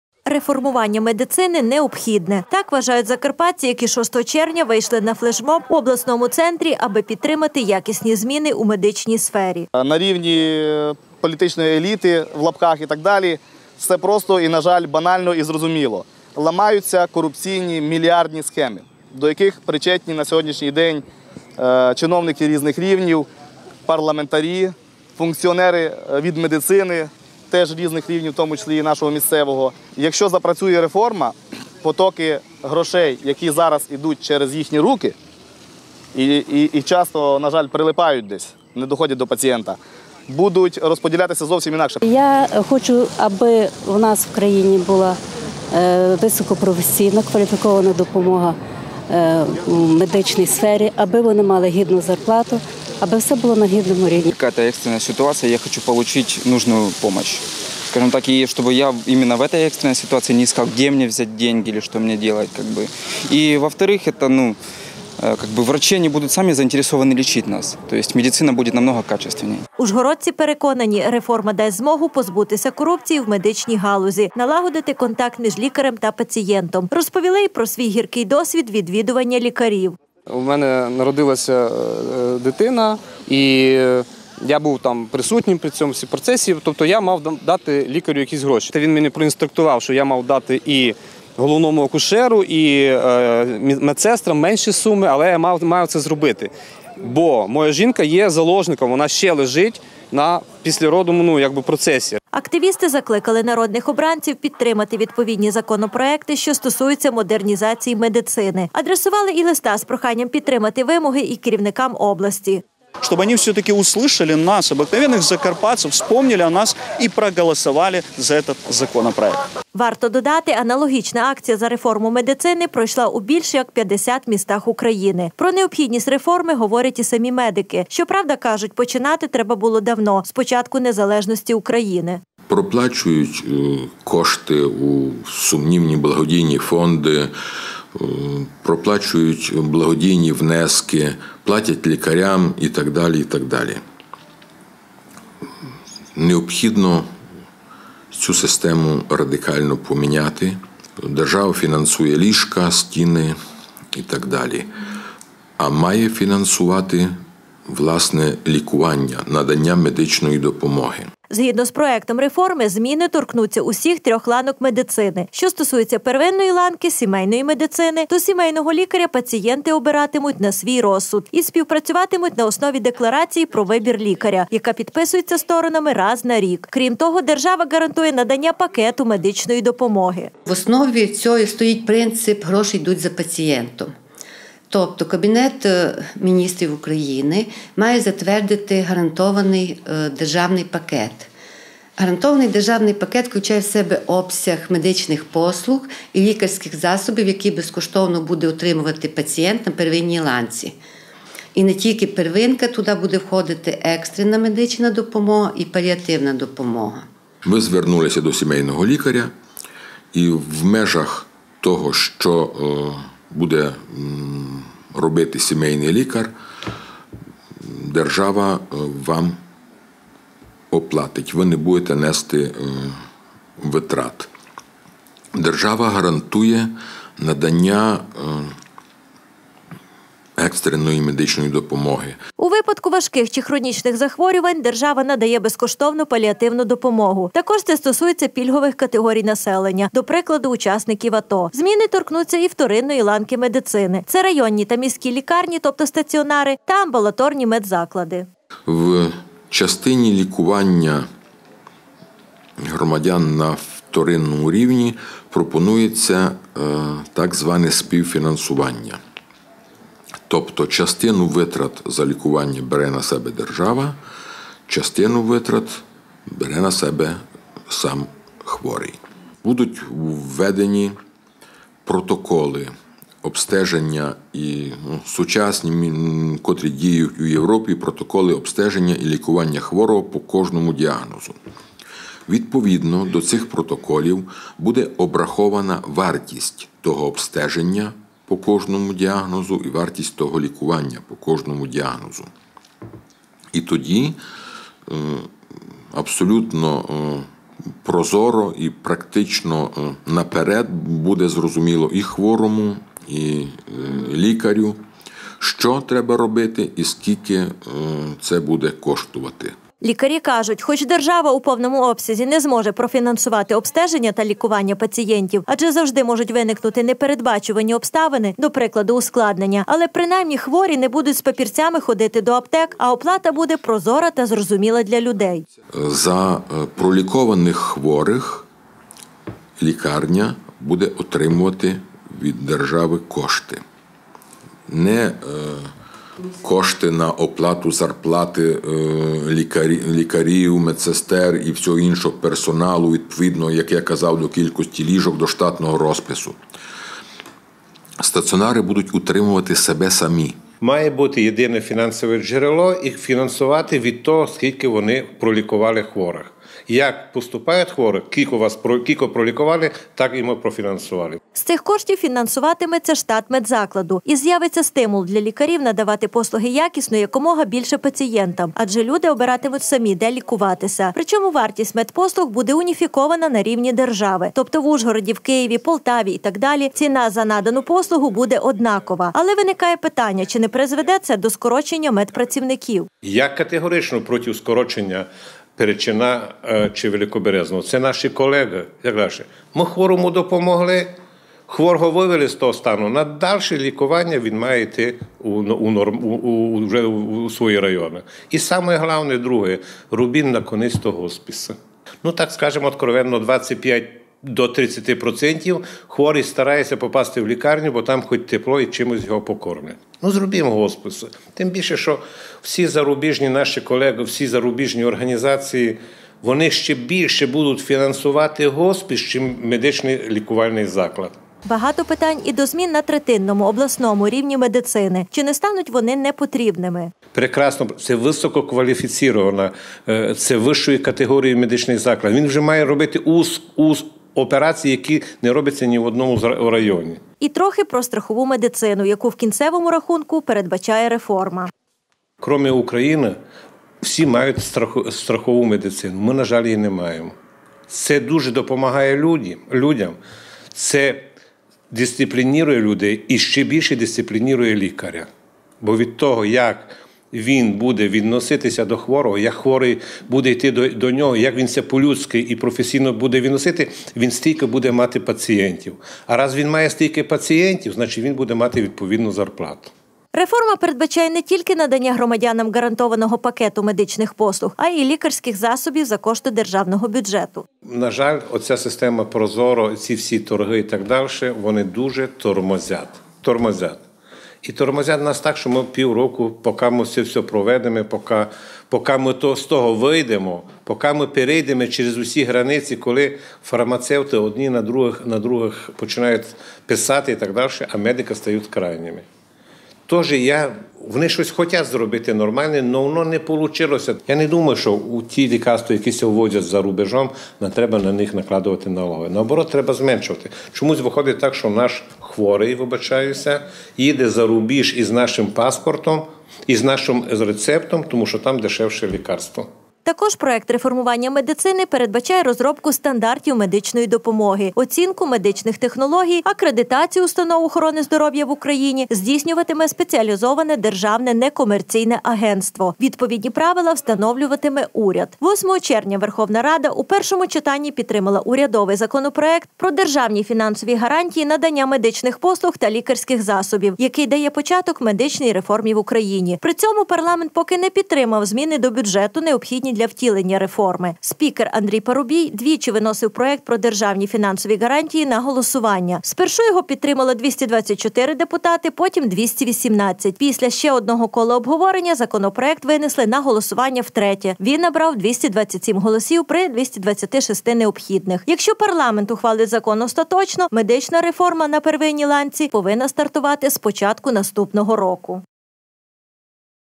Старт медичної реформи
Верховна Рада дала старт мед реформі, у першому читанні підтримала урядовий законопроект про державні фінансові гарантії надання медичних послуг та лікарських засобів, який дає початок медичній реформі в Україні. Якщо парламент ухвалить закон остаточно, медична реформа на первинній ланці повинна почати діяти з початку наступного року. Реформування медицини необхідне! – так вважають закарпатці